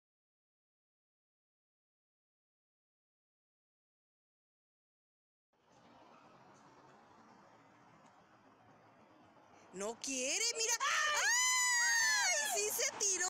Se lanza de puente vial en San Pedro | Monterrey
Un hombre resulta con lesiones graves al lanzarse de un puente vehicular de la Avenida Morones Prieto, en San Pedro.